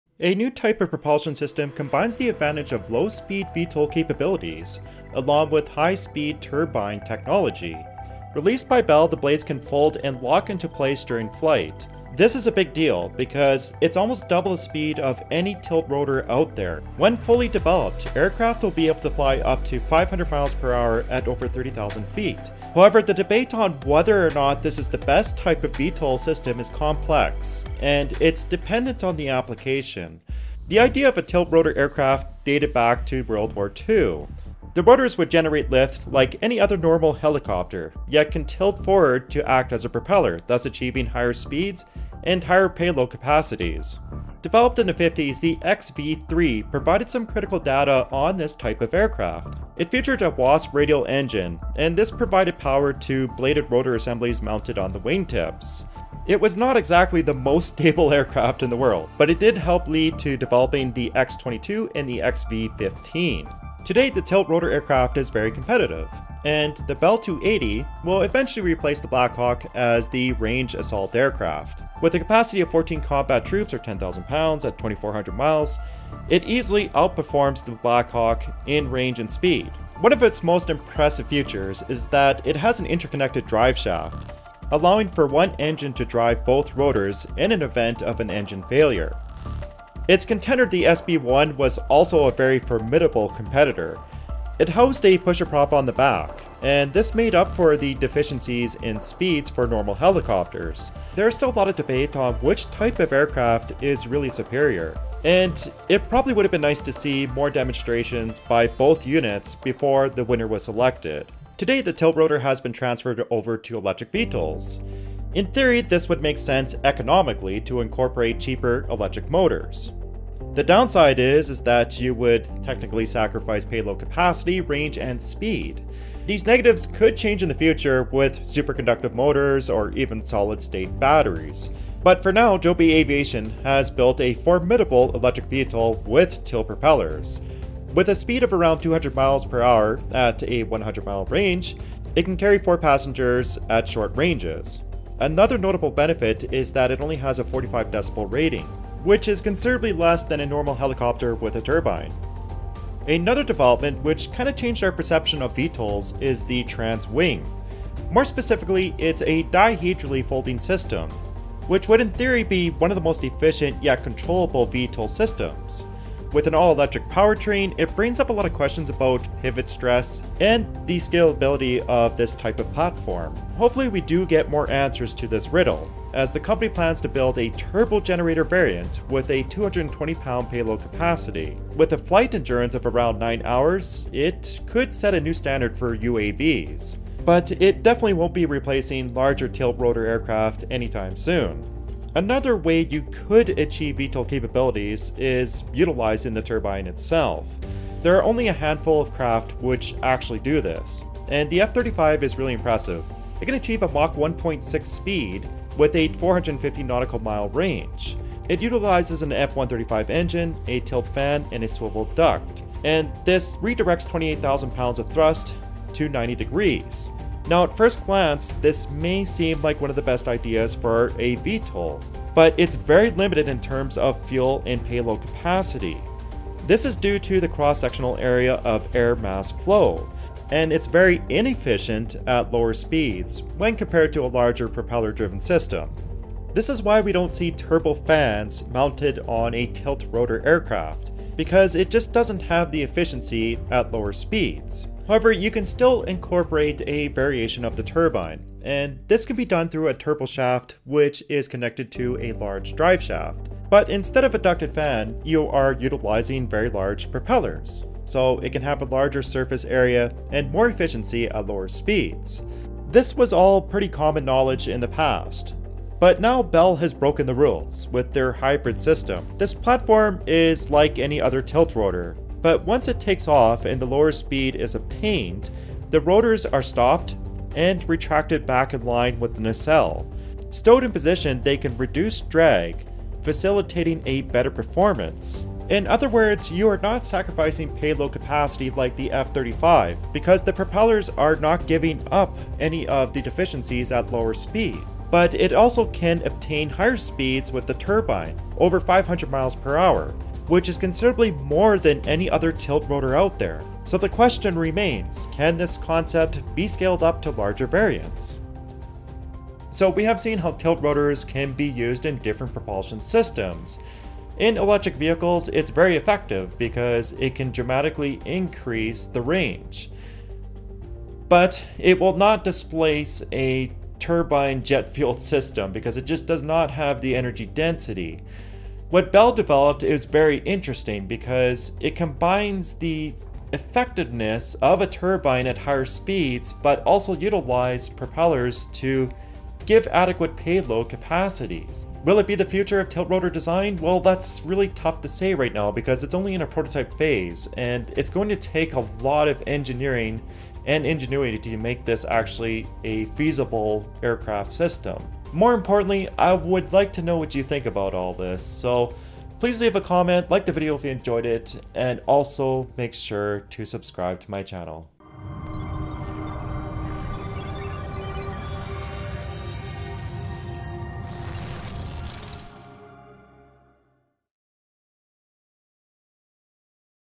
HYBRID Jet Engine Will Change AVIATION Forever
HSVTOL technology blends the hover capability of a helicopter with the speed, range and survivability features of a jet aircraft. Bell’s scalable family of HSVTOL aircraft systems is critical to future mission needs and serving the next generation of warfighters.